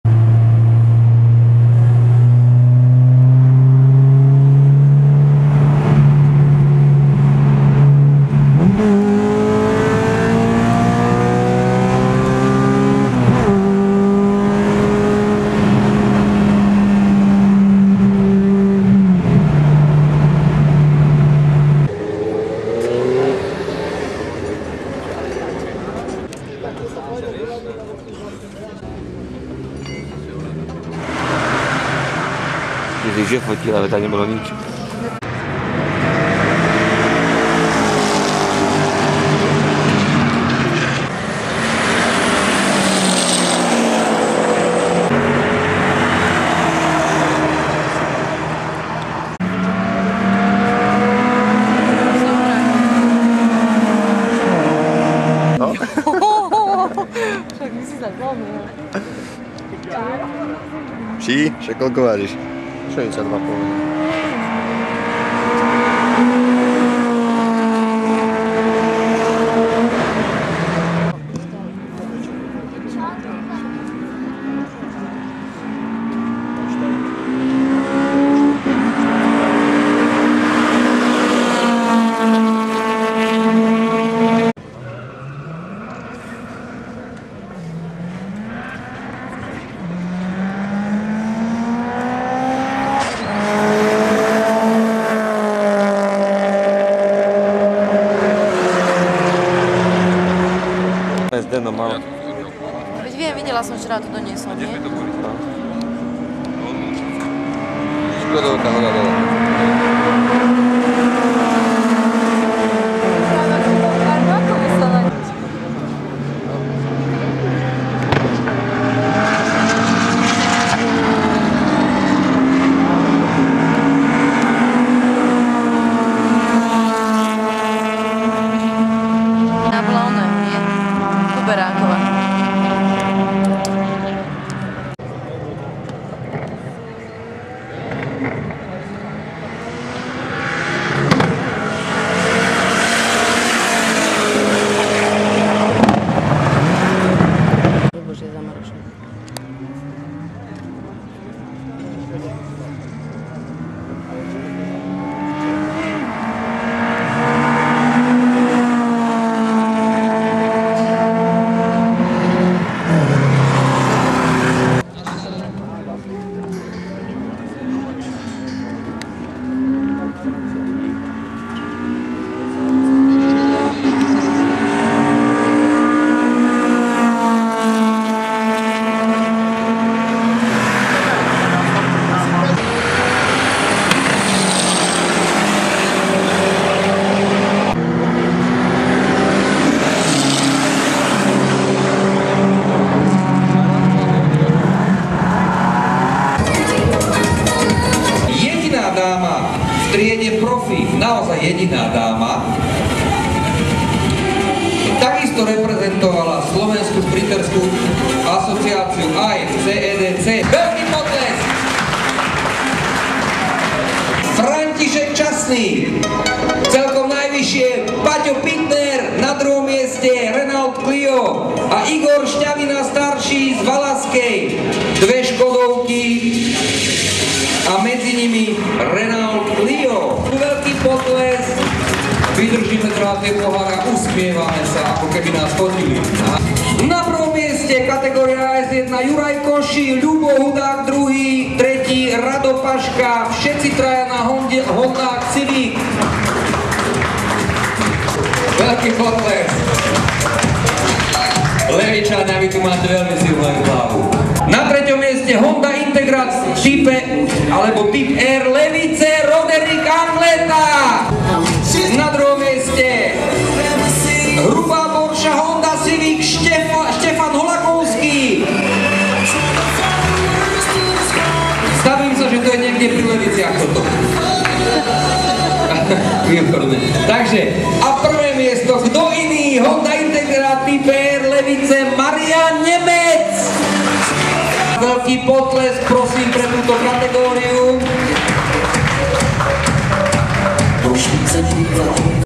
SSA zrychlenie.sk, odovzdavanie titulov 2010´
16.október 2010 Slovakiaring -- Orechová Potôň, Koncoročné vyhlásenie víťazov Slovenského šprintérskeho šampionátu 2010

Zoznam ocenených pretekárov:
Autá trieda PROFI:
P1: 1. Mátyás Katona, 2. Ján Černý, 3. Erik Sládek
P2: 1. Ladislav Pakši, 2. Norbet Kiss, 3. Tamás Kocsis
P3: 1. Gyetvai Miklós, 2. Tomás Polonec, 3. Juraj Matlovič
P4: 1. Pavol Buček
P5: 1. Tibor Eotvos, 2. Peter Pap, 3. Zdeno Hubka
P6: 1. Monika Weisová, 2. Roland Kovács

Autá trieda STREET:
S0: 1. František Časný, 2. Patrik Pittner, 3. Igor Šťavina
S1: 1. Juraj Kossi, 2. Ľuboš Hudák, 3. Radoslav Paška
S2: 1. Marian Nemec, 2. Štefan Holakovský, 3. Roderik Angleta
S3: 1. Štefan Szalai, 2. Róbert Molnár
S4: 1. Oliver Čurgali, 2. Miroslav Kapitulčin
S5: 1. Pepa Jonea, 2. Vladimír Kubáň, 3. Ján Banas
S6: 1. Ľuboš Čelín, 2. Branislav Farkaš, 3. Gustáv Loška

MOTOCYKLE:
M1: 1. Karel Sojka
M2: 1. Mačai Radovan, 2. Ján Lalík, 3. Pavol Sedliačik
M3: 1. Jozef Horváth, 2. Tomáš Ronai, 3. Štefan Kubáň
M4: 1. Stano Poživenec, 2. Branislav Farkaš, 3. Miroslav Havala
M5: 1. Robert Fejt, 2. Erik Szalai, 3. Michal Kadlec